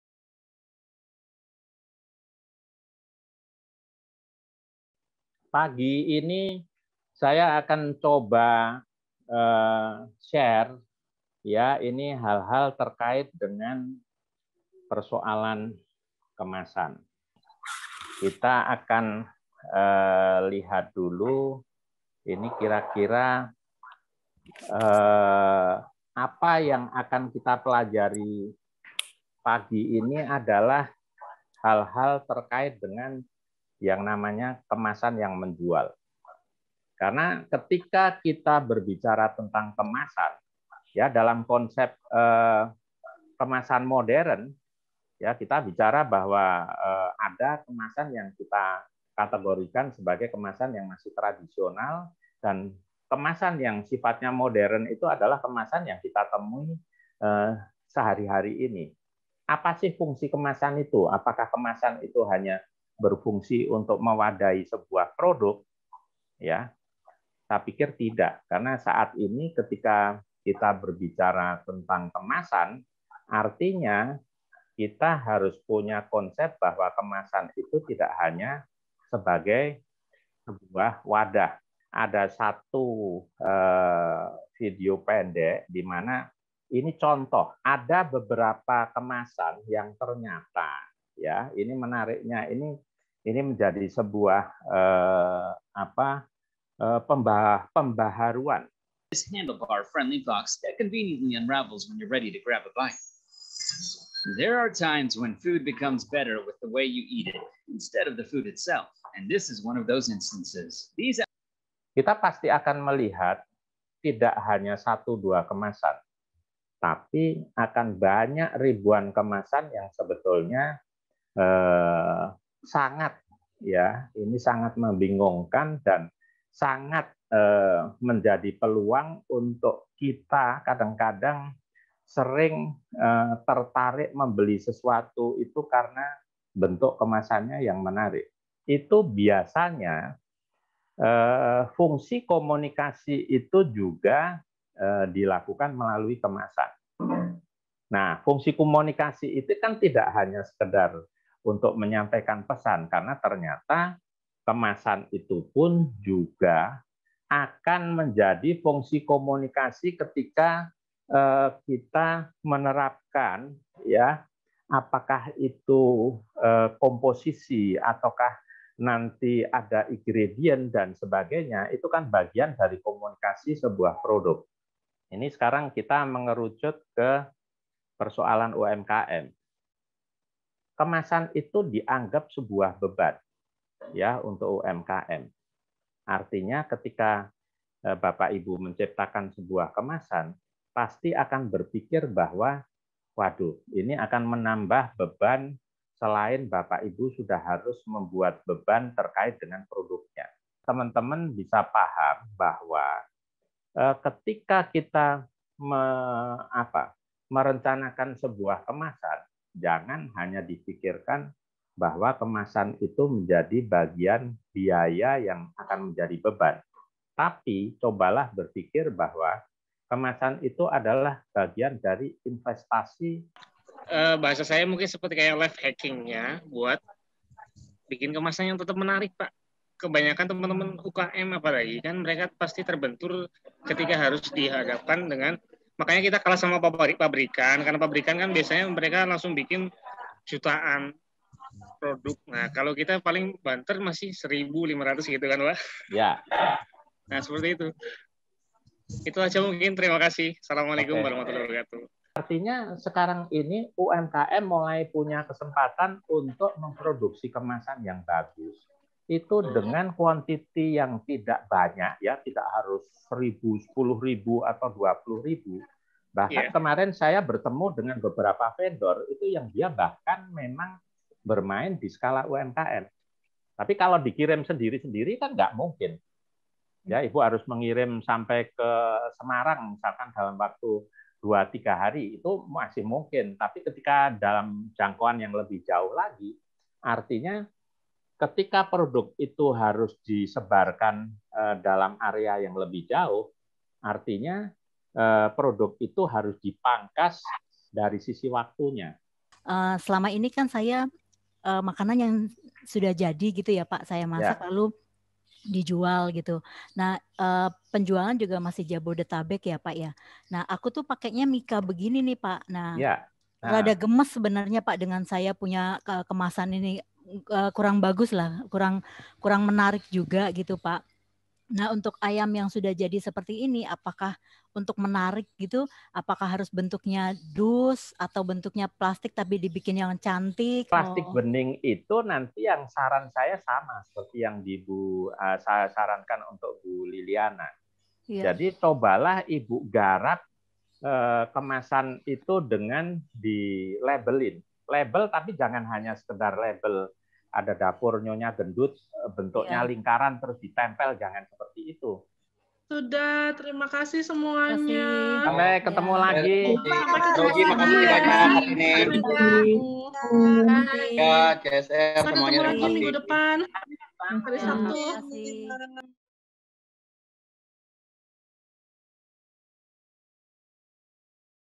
Pelatihan UMKM - CSR Bidang Ekonomi (Kemasan Yang Menjual)
Sabtu ini, Pelatihan UMKM kembali diadakan secara online bersama Narasumber Bapak Rujiyanto dengan topik yang sangat menarik "Kemasan yang Menjual" topik yang terlihat sederhana namun merupakan permasalahan bagi sebagian besar binaan, terutama pelaku UMKM.

Masih sedikit pelaku UMKM yang menyadari bahwa Kemasan merupakan salah satu pemikat utama dalam menjual produk dan juga banyak orang yang masih ragu untuk berinvestasi membuat kemasan produknya menjadi lebih menarik dan mampu mengundang orang-orang yang melihatnya untuk membeli.

Lewat diskusi dan tanya jawab yang diadakan, terlihat betapa antusiasnya para peserta untuk belajar dan memikirkan bagaimana caranya merubah kemasan produknya supaya lebih menjual.

Semoga pelatihan hari ini bisa merubah wajah kemasan produk para binaan menjadi lebih menarik dan tentunya dengan tidak mengabaikan kualitas produk itu sendiri... sehingga bisa meningkatkan omset penjualan produknya.

Salam semangat, maju bersama CSR bidang Ekonomi Sinarmas Land